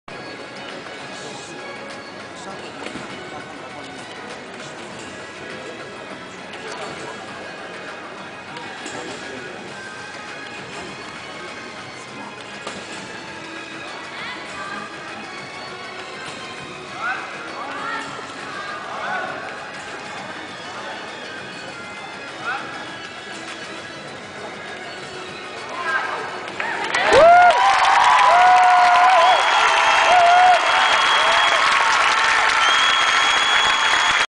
Emma Smith - Team Final - Trampoline World Championships 2011
28th Trampoline and Tumbling World Championships
17th-20th November 2011
National Indoor Arena, Birmingham

Score for this routine: 53.325

Final Result:
Gold - China
Silver - Great Britain
Bronze - Canada